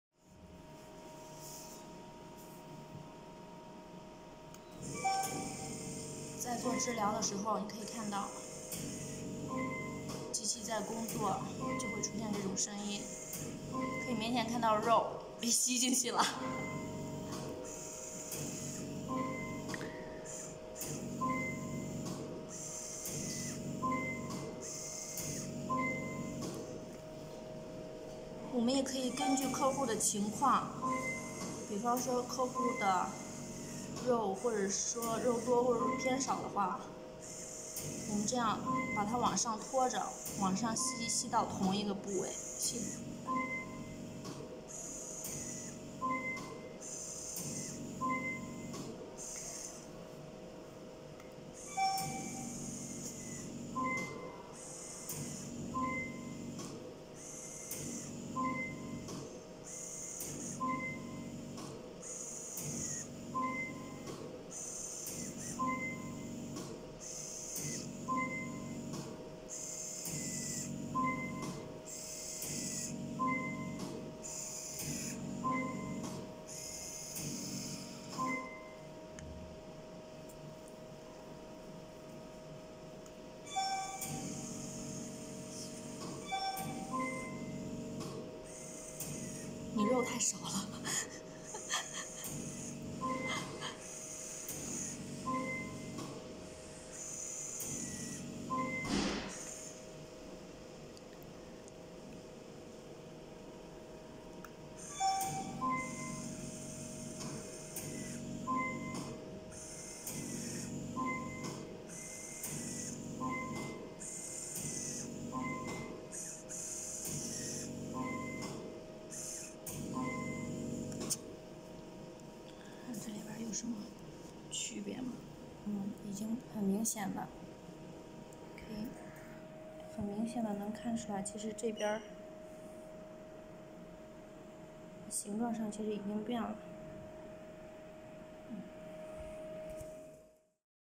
Velashape treatment
1. This machine has a different mode for different parts, it has guide mode and guide time for best effect and best operation,
2. RF max temperature can achieve 50 Temperature, can adjustable, makes patients comfortable.
3. IR, Vacuum, and RF has 3 levels, you can set it by yourself.
4. have guide mode and guide operates on screen, you can easily to study and bring you the best effect.